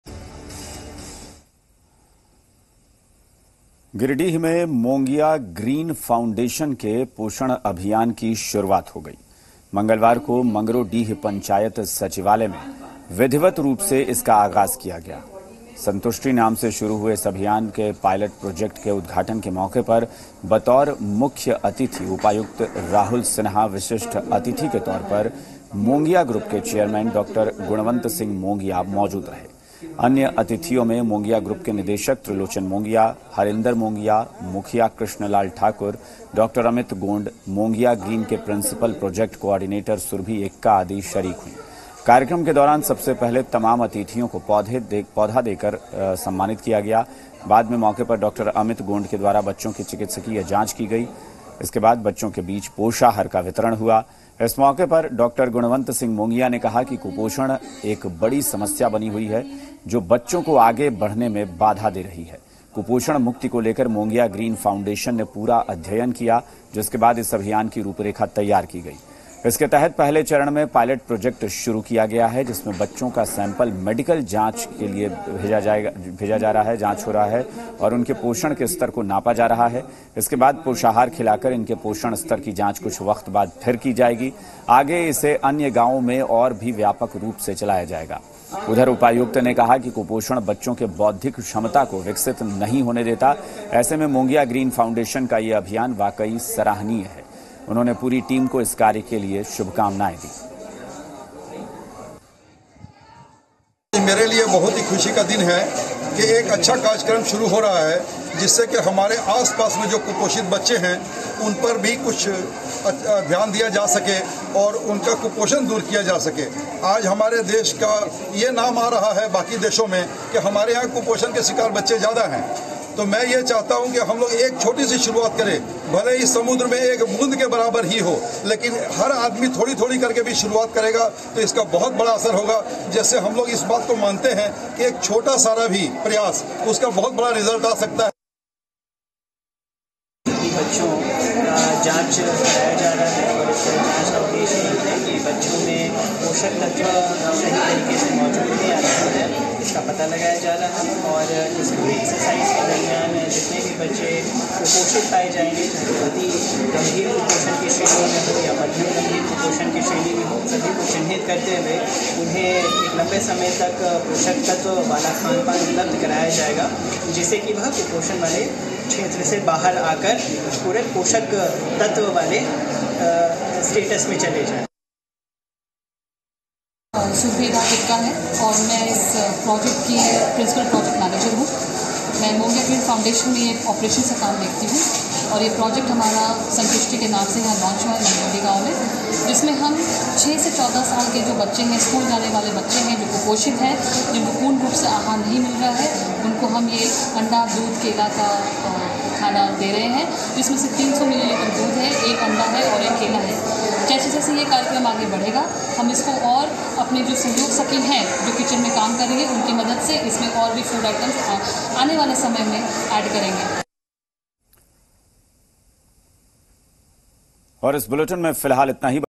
गिरिडीह: कुपोषण से लड़ने के लिए पोषण अभियान 'संतुष्टि'
गिरिडीह में मोंगिया ग्रीन फाउंडेशन के पोषण अभियान 'संतुष्टि' की शुरूआत हो गई. मंगलवार को मंगरूडीह पंचायत में विधिवत रूप से इसका आगाज हुआ. इस मौके पर गिरिडीह के उपायुक्त राहुल सिन्हा और मोंगिया ग्रुप से चेयरमैन गुणवंत सिंह मोंगिया मौजूद रहे.